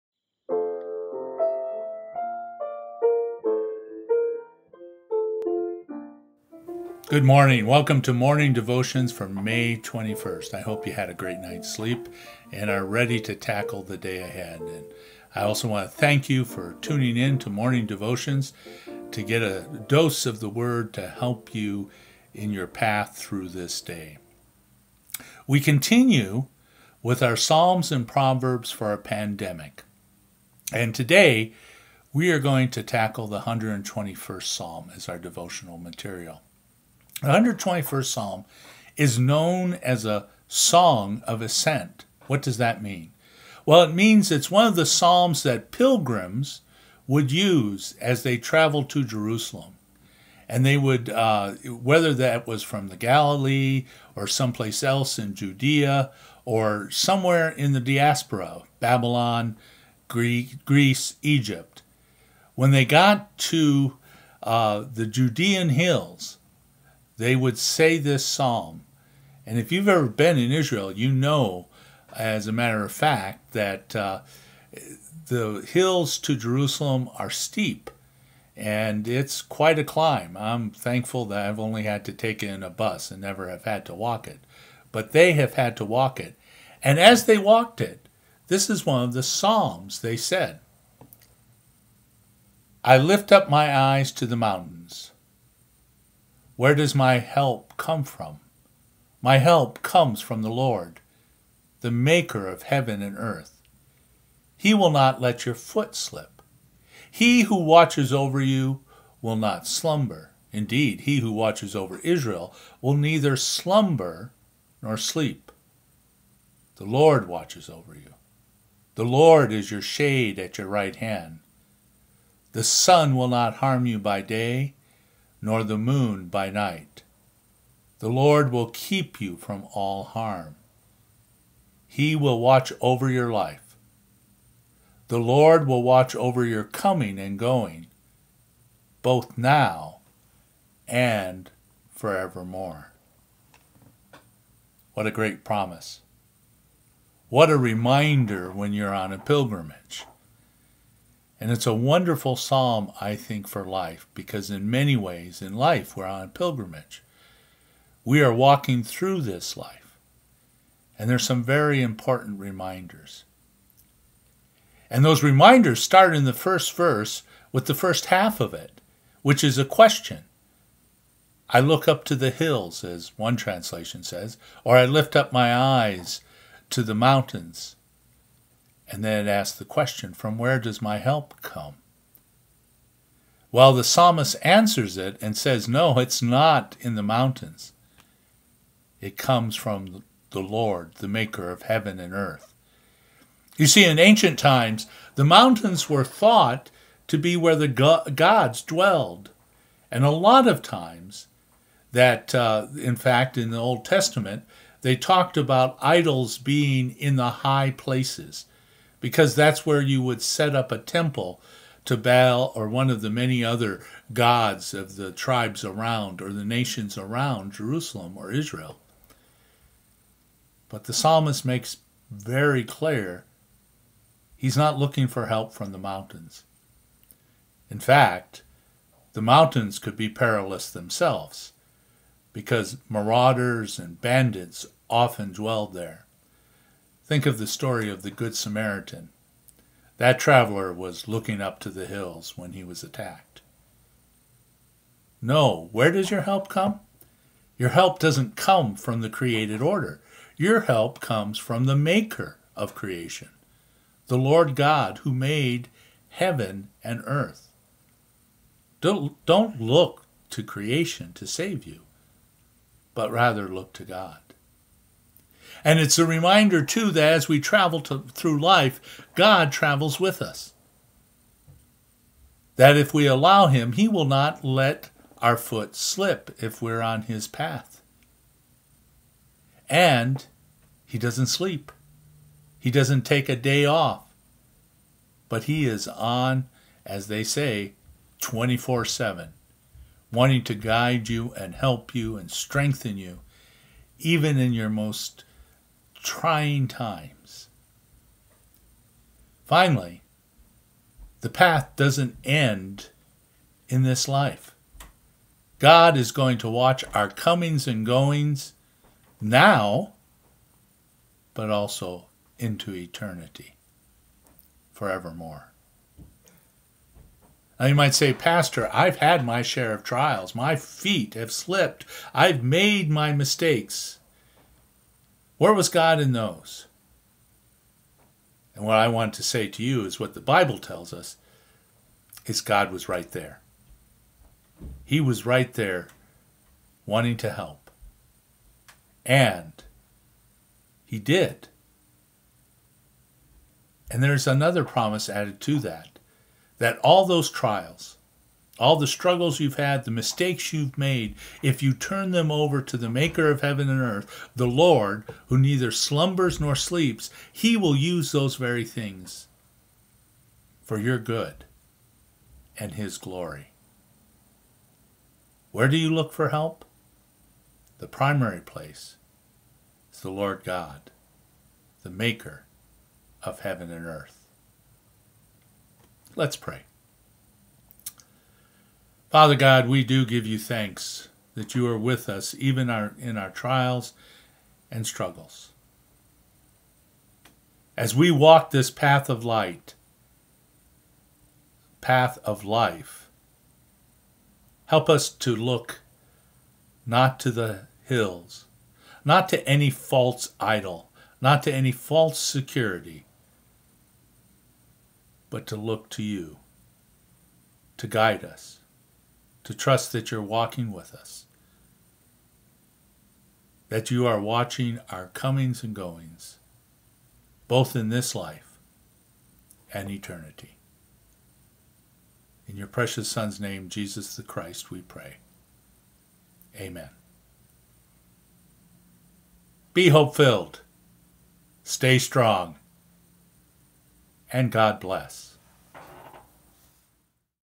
Devotions 5 21
Pastor Carl looks at Psalm 121 and asks the question, "Where does our help come from?"